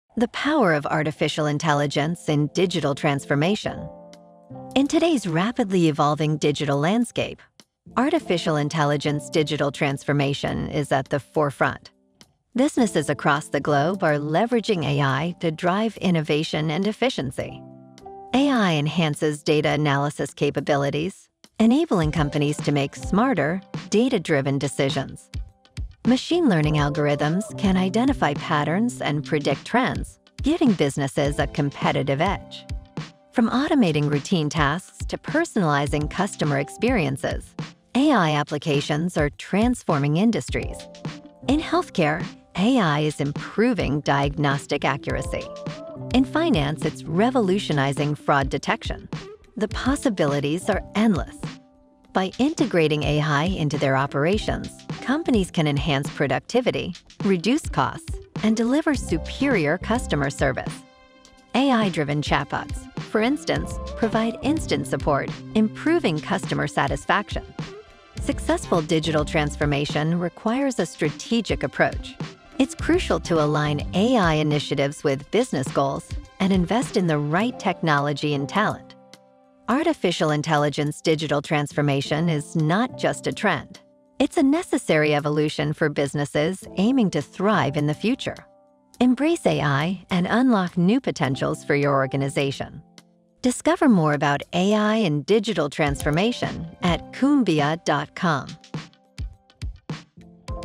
The Power of Artificial Intelligence in Digital Transformation
Artificial Intelligence digital transformation is not just a trend; it's a necessary evolution for businesses aiming to thrive in the future. Embrace AI and unlock new potentials for your organization.

We're AI experts who can help you craft the perfect AI for your business.